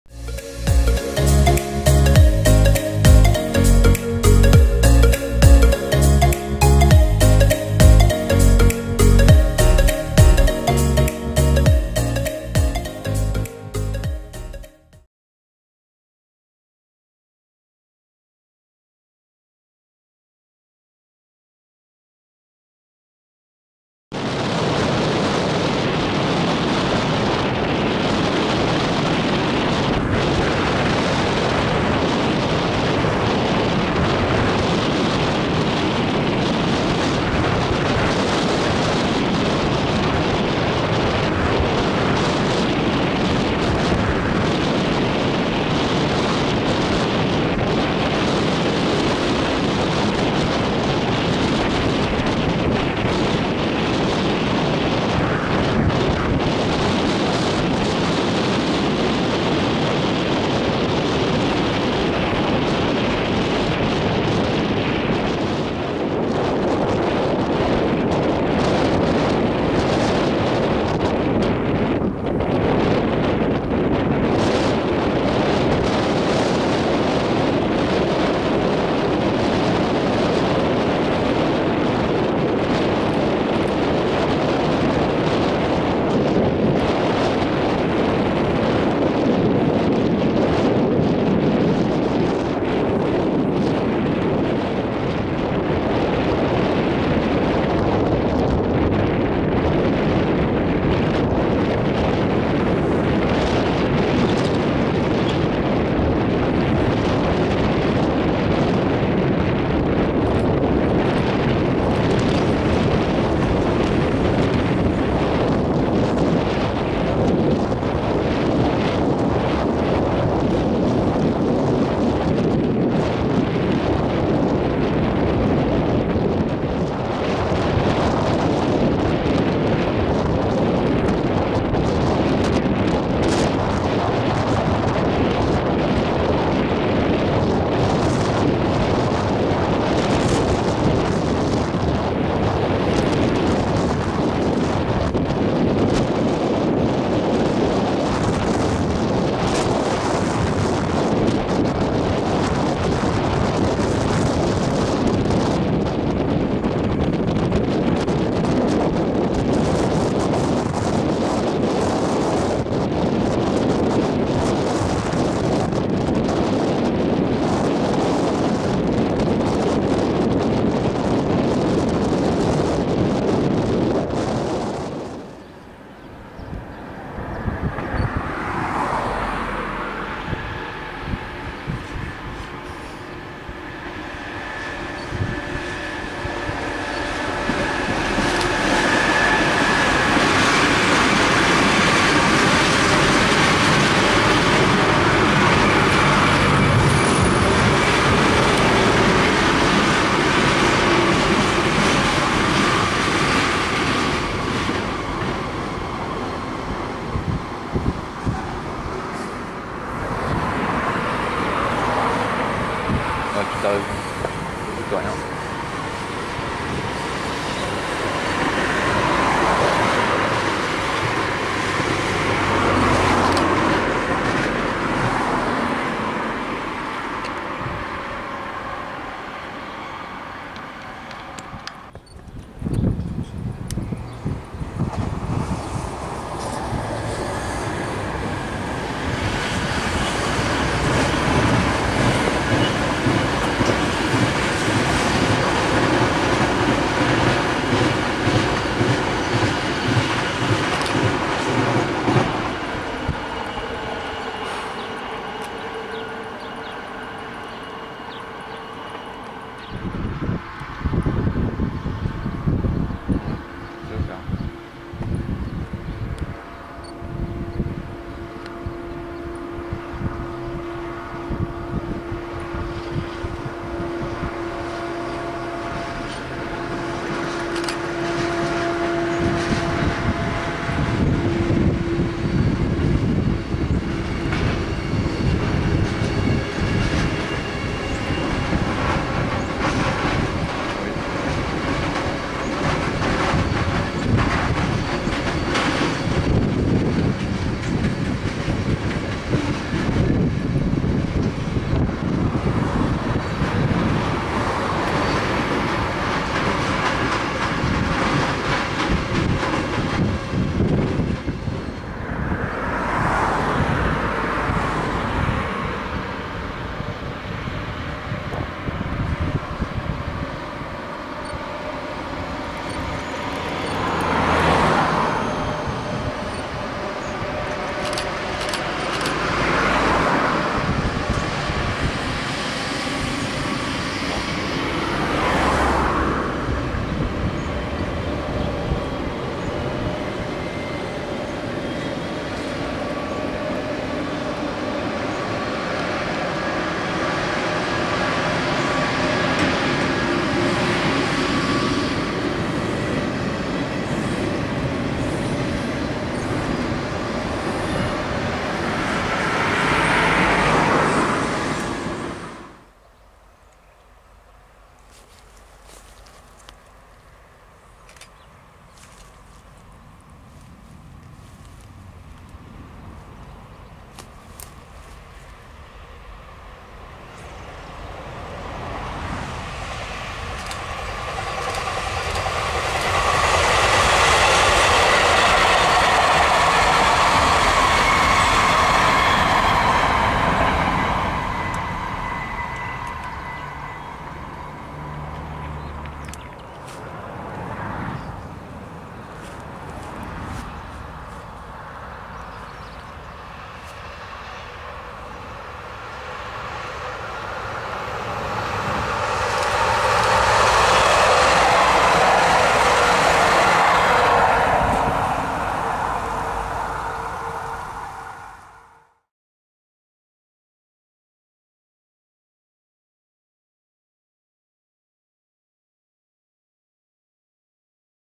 Des trains sur les rails d'Occitanie
Bonjour,
On se retrouve avec une vidéo tournée en Occitanie. La première partie de la vidéo est tournée à Port-la-Nouvelle sur la ligne Perpignan - Narbonne, on y trouve en particulier l'acheminement par la BB 22319 d'un TGV Ouigo pour l'Espagne, le reste est classique (TGV, AGC, BB 27000, TRAXX). La deuxième partie de la vidéo est tournée à Vernet sur la ligne Toulouse - Latour-de-Carol, avec en particulier un FRET en TRAXX ECR.
Bon visionnage !